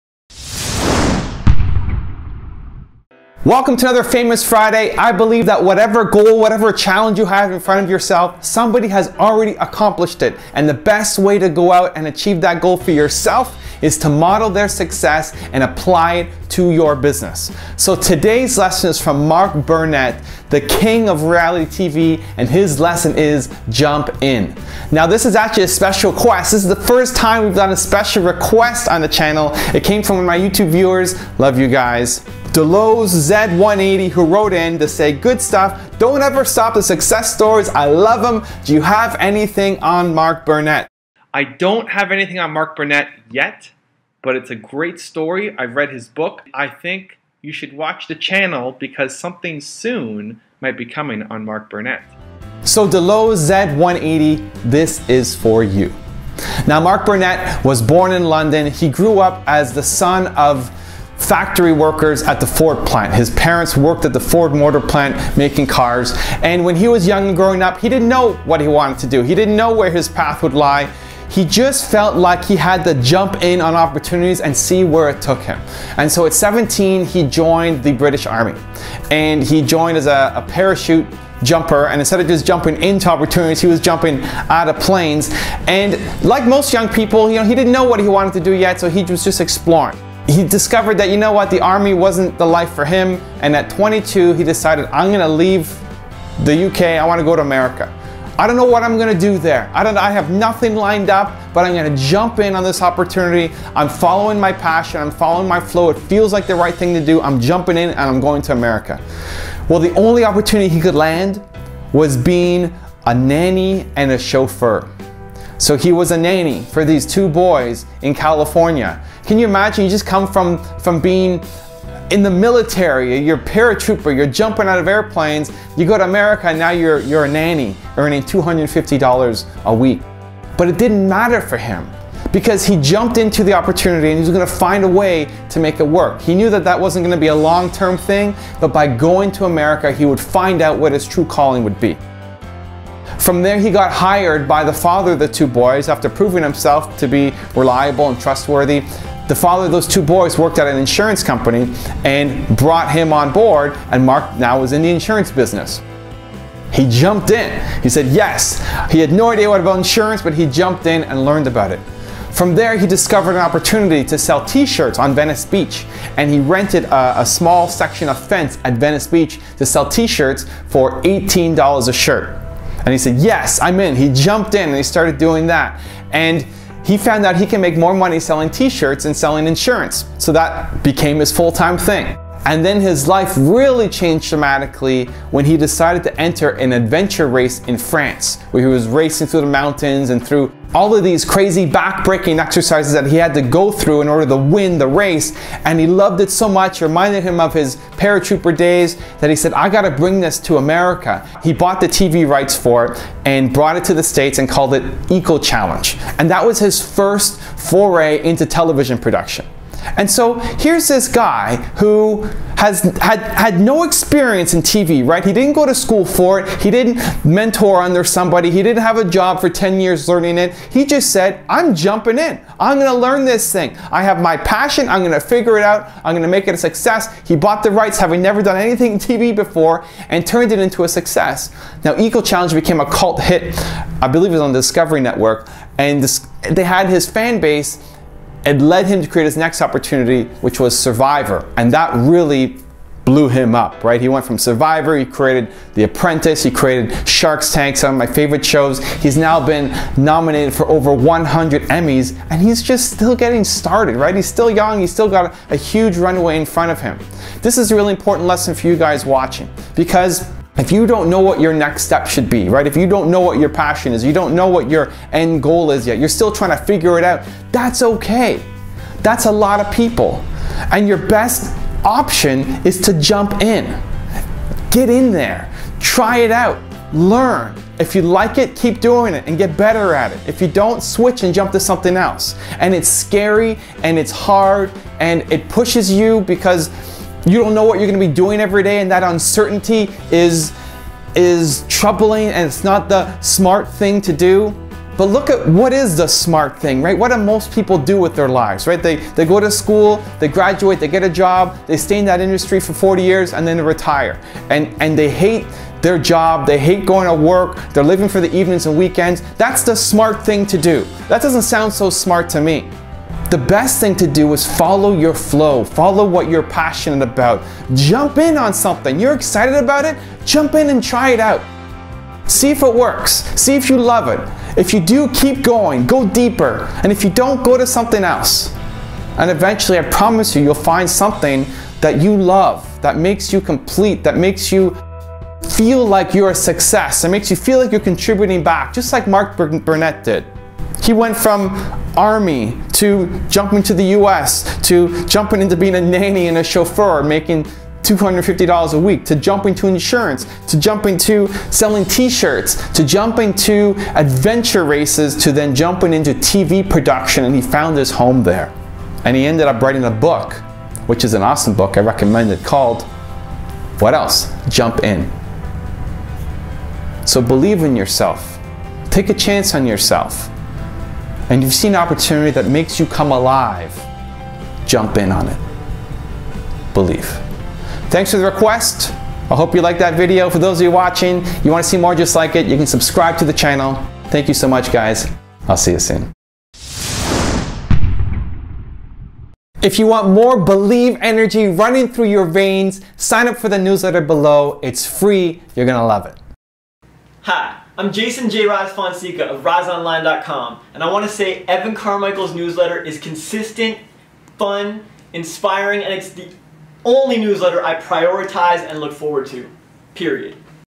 Mark Burnett: If You Want To Know How To Turn Challenges Into Opportunities, Try THIS!
* Your Questions - Every day!